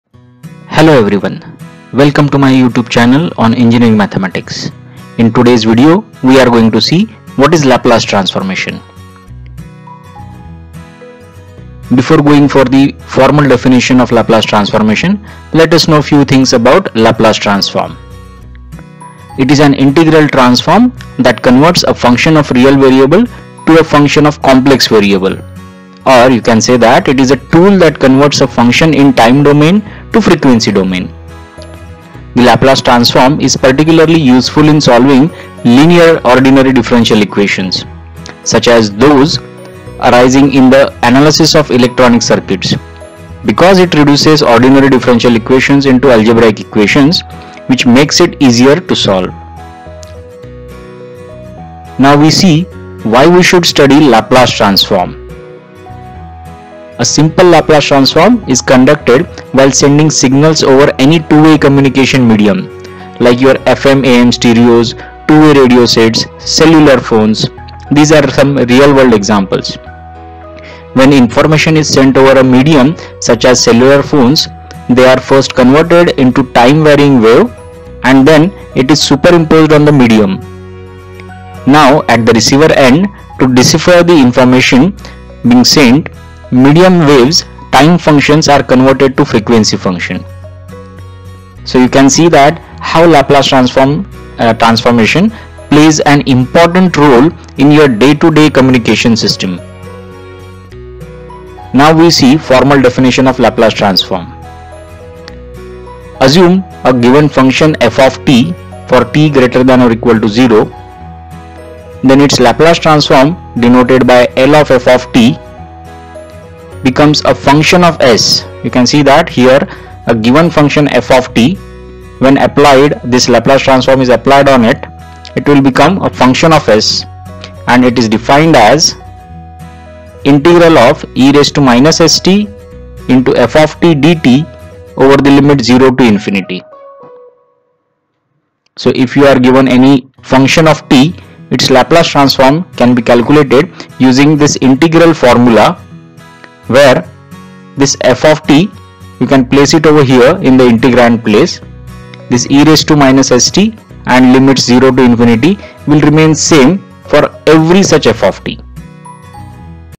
Laplace Transformation definition
Students of engineering mathematics will able to understand what is Laplace transformation means. Its formal definition is discussed here. Real life use of Laplace transformation in communication system is given in this video.